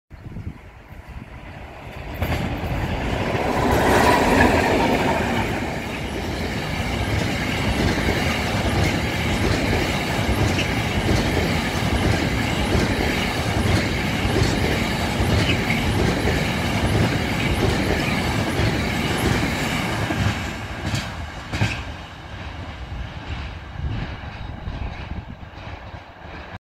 TEM2-042B z platformami
TEM2-042B z platformami mija Jarocin w kierunku Poznania